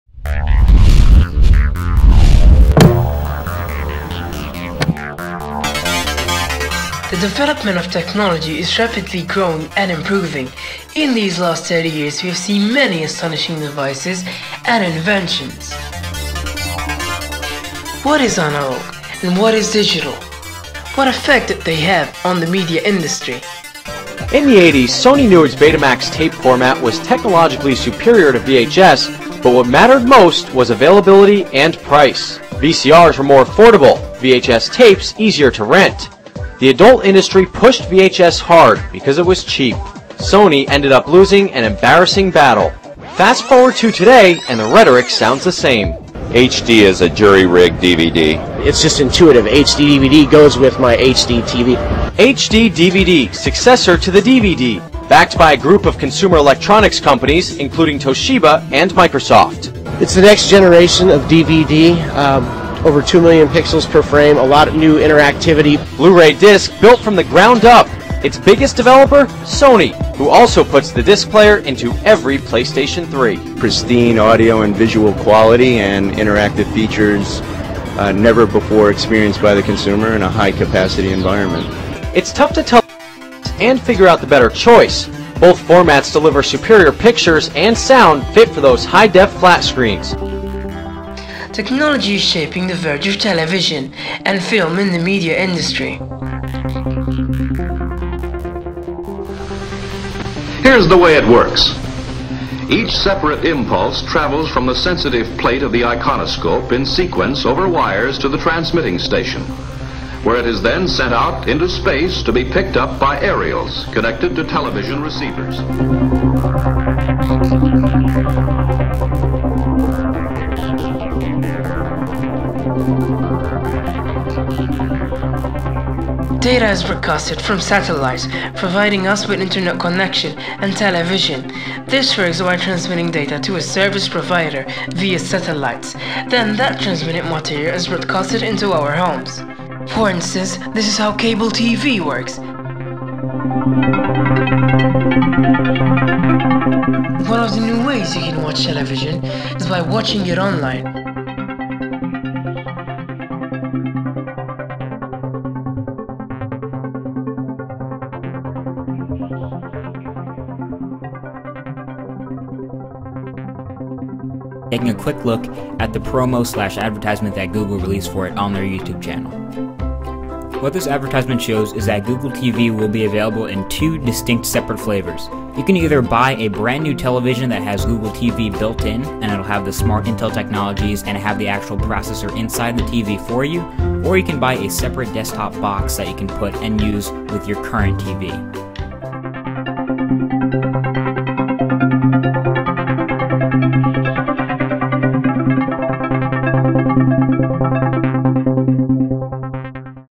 Technology of the Past VS Technology of The Future
Here is a 3 minute video that has been created for a school assignment. This video shows a change which technology created in the film and TV  industry and how nowadays technology keeps on influencing the world of media. All visual and audio content synchronised with each video were found online. For does who are interested in any of the content shown in this project you can find a link in the end of this 3 minute video for each image and video used.